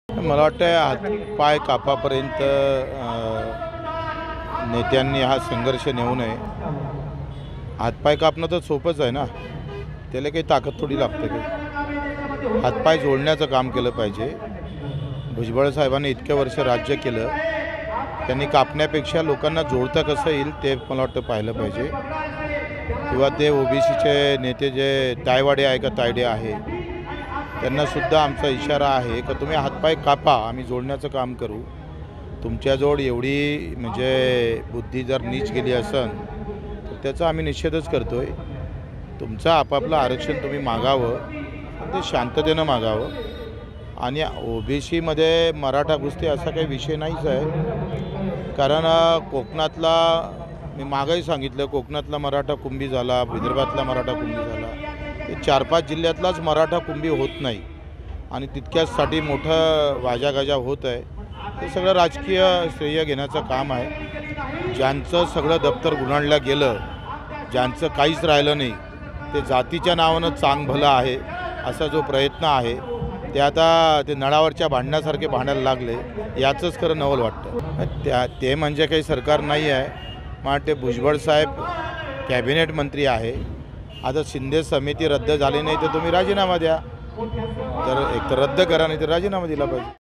Bacchu Kadu Vs Bhujbal | शिंदे समिती बरखास्त करण्याच्या विधानावर बच्चू कडू यांचे भुजबळांना आव्हान
छगन भुजबळ राजीनामा द्या: न्यायमूर्ती शिंदे समिती बरखास्त करण्याच्या विधानावर बच्चू कडू यांचे भुजबळांना खुले आव्हान